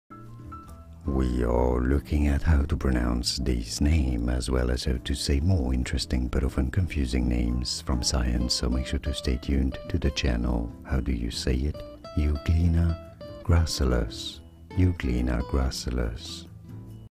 How to Pronounce Euglena Gracilis
This video shows you How to Pronounce Euglena Gracilis, pronunciation guide.

00:00 - Pronunciation Intro
00:08 - How to Pronounce Euglena Gracilis